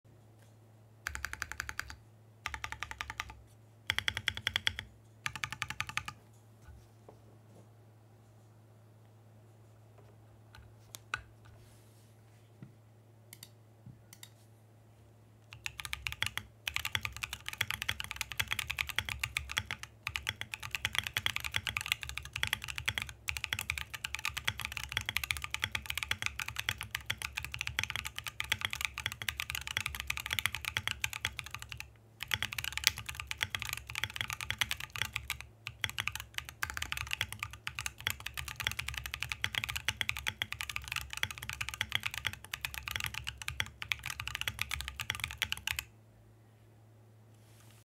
Portal65 | Cheesecake
I have not been loving this board so much ever since obtained it. But I think I have designed a switch that just saved my portal.

Pre-order for the switches are also live at various vendor world-wide.

Board specs:
Plate: FR4
Mount: O-ring mount
Foam:  No Foam
Keycap: MW Rubber X-mas. PBT Dye-sub

Switch:
Top housing: POK
Bottom housing: POK
Stem: LY
Spring: dual-stage 60g. Bottom out 58g
Factory lube: Yes

**Recorded with iPhone14 Pro Max and Elgato Wave 3**